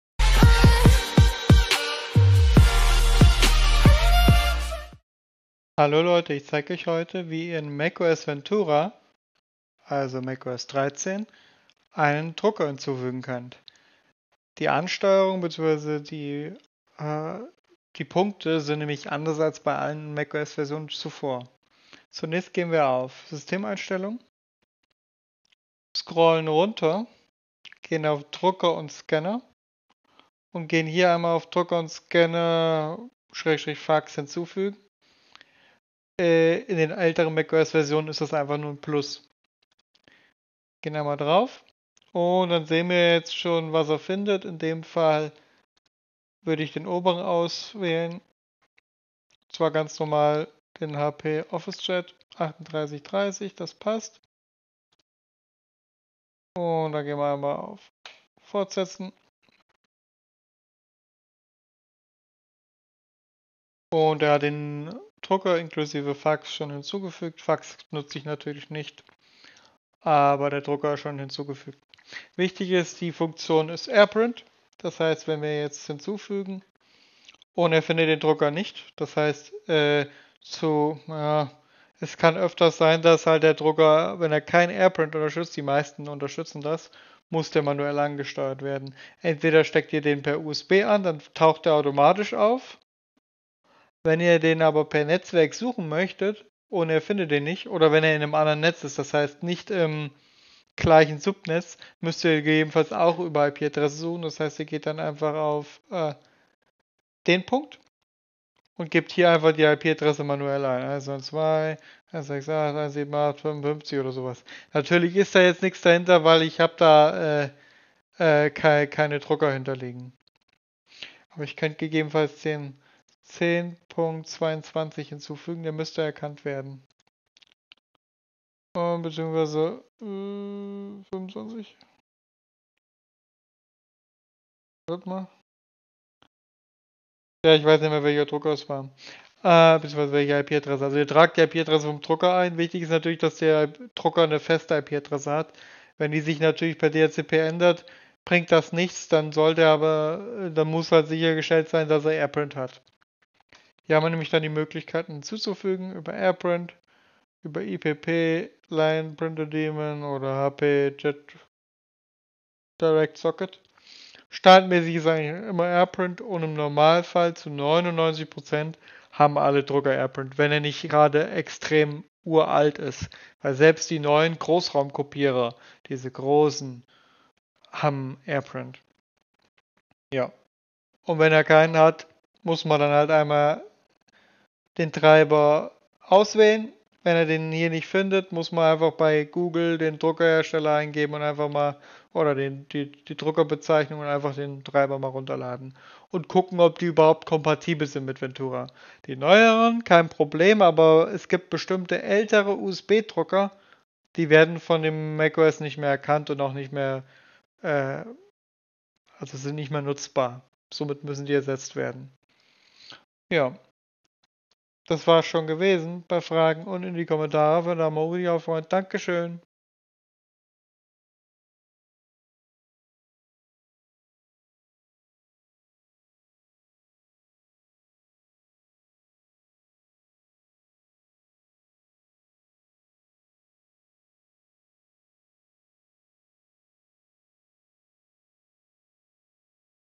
Neuen Drucker in macOS Ventura hinzufügen | Marcel Scholz
Hallo Leute,

ich zeige euch heute wie ihr in macOS Ventura einen neuen Drucker hinzufügen könnt. Die Positionen der Systemeinstellungen haben sich in macOS Ventura geändert.

MARCEL SCHOLZ